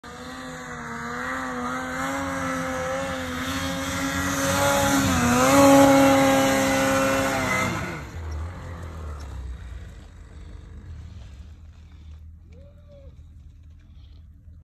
Should we head back to this spot again this year?
Mountain riding in Revelstoke BC Canada March 2022